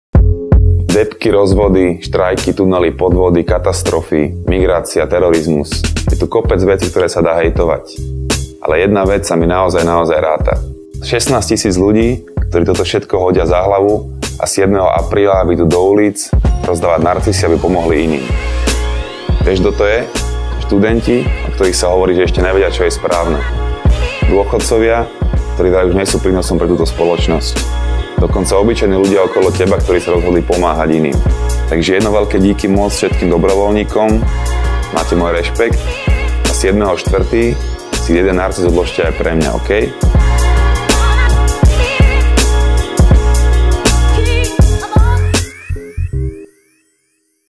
Slovenská sporiteľňa - Deň narcisov 2017
Podporte spolu s nami Deň narcisov, jedinú verejno-prospešnú zbierku Ligy proti rakovine. Viac sa dozviete na